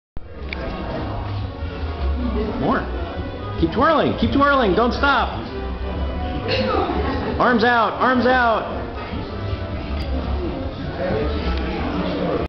Mackenzie twirling
Mackenzie twirling at Janet's 70th birthday party